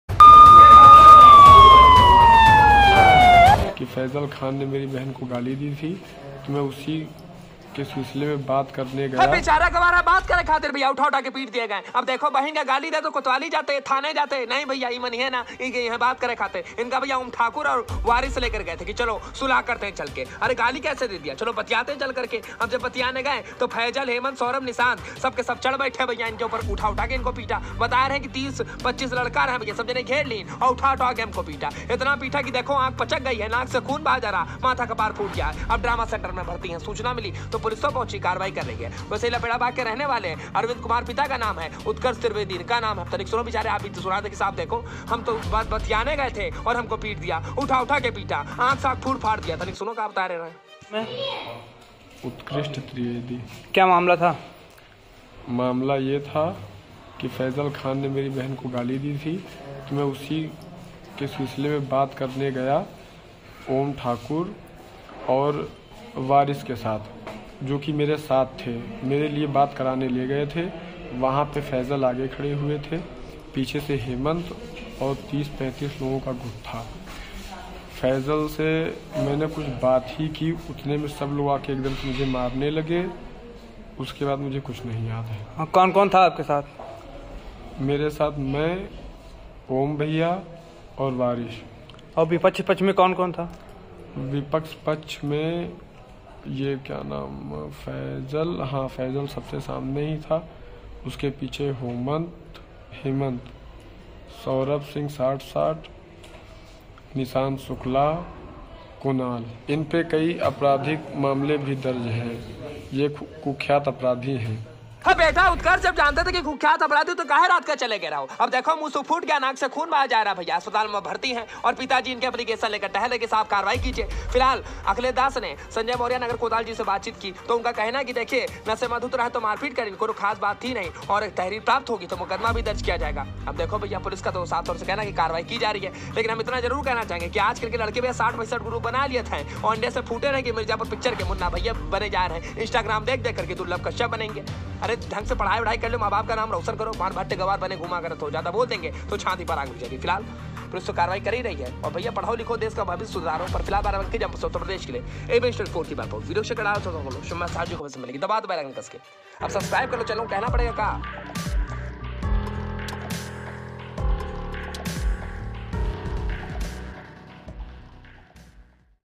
लौंडे बन रहे मुन्ना भैया ! Barabanki में युवक को उठा उठाकर पटका दे दना दन अस्पताल में भर्ती !
लखपेड़ाबाग के रहने वाले अरविंद के लड़के उत्कर्ष त्रिवेदी के. सुलह कराने के नाम पर माल गोदाम रोड पर बुलाया उसके बाद दे दना दन शुरू... फैजल, हेमंत, सौरभ, निशांत, समेत 30-25 लड़के.